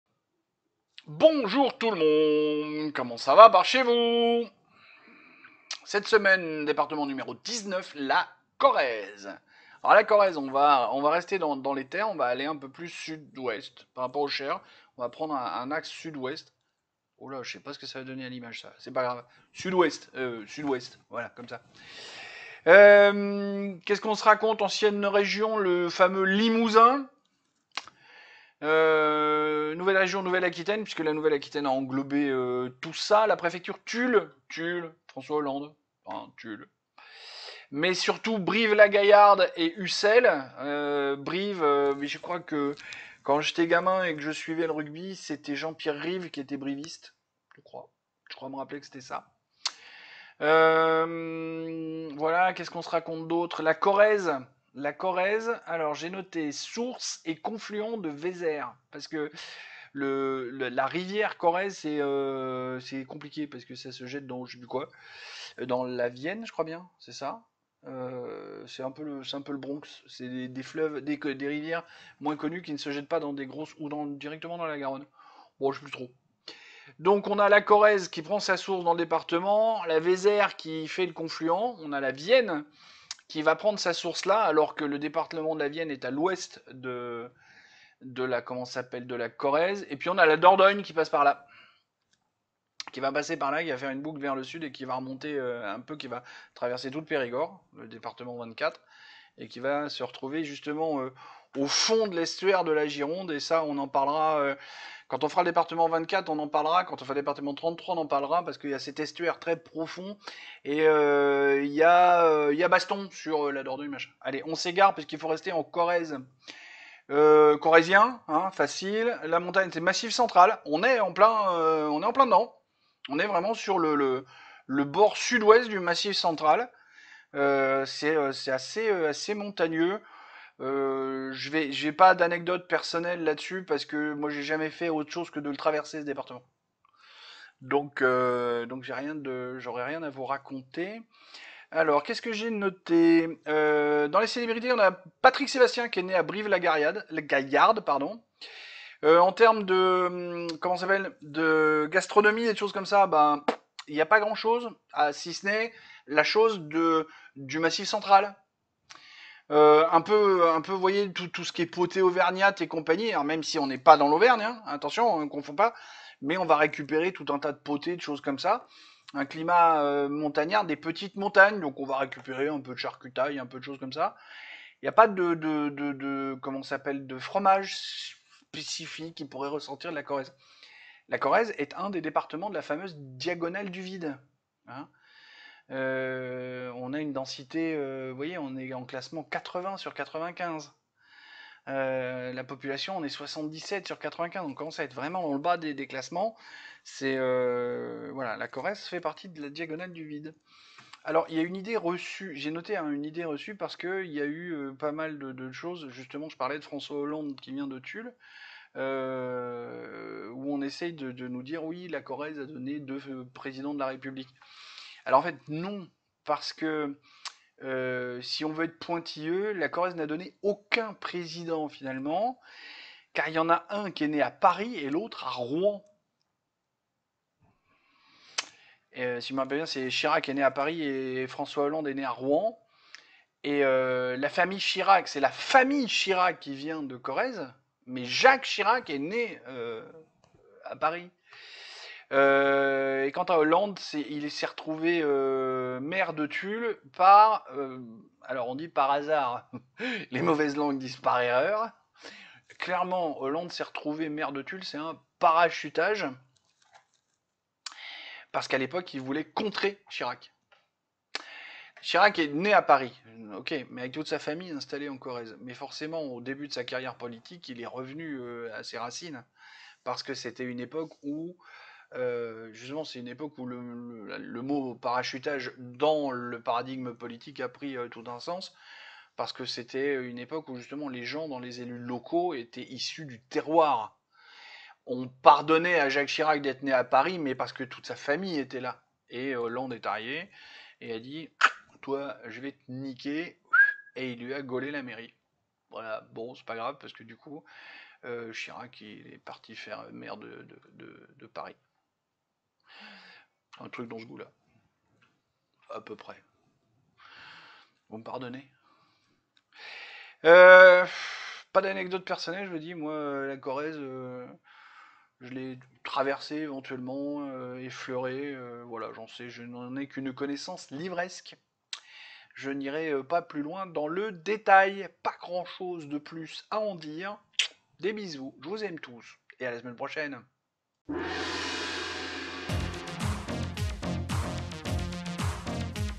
#019 Les départements Corrèze
Cette semaine la Corrèze, dix-neuvième département Français de la liste.